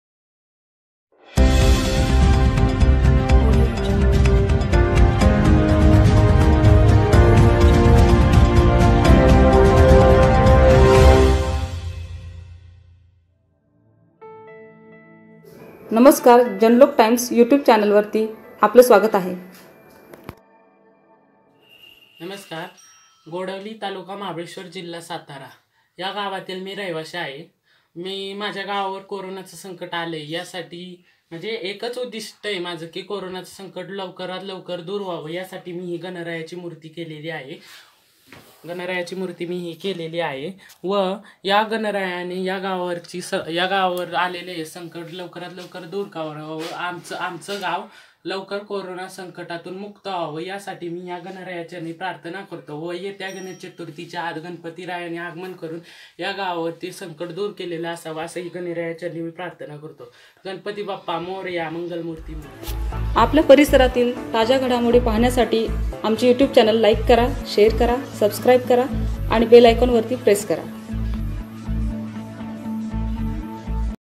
जनलोक टाईम्स पांचगणी येथे मुलाने स्वतःगणपती बनवून कोरोना हद्दपार व्हावा यासाठी गणरायाला केली विनवणी
घरी रहा सुरक्षित रहा.